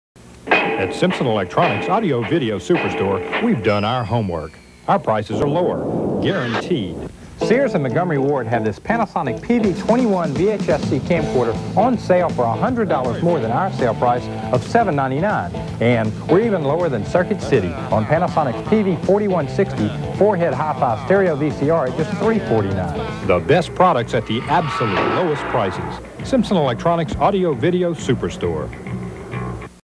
Simpson Electronics Audio and Video Superstore ad 1992 Alexandria
Here is a 1992 electronics ad from Simpson Electronics Superstore in Alexandria, LA on Macarthur Drive.  Featuers Panasonic products.